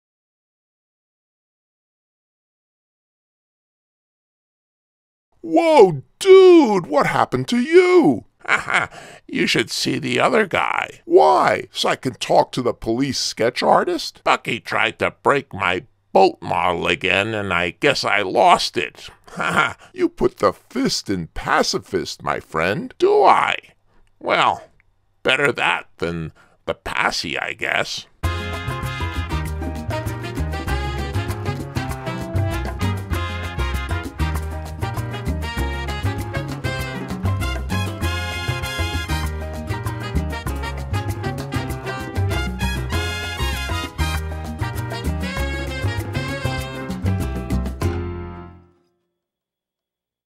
Get Fuzzy by Darby Conley Published Saturday, April 25, 2020
It was wartime and Mayor Fiorello LaGuardia read the comics on the radio, here now is my YouTube comic #80 Get Fuzzy by Darby Conley Published  Saturday, April 25, 2020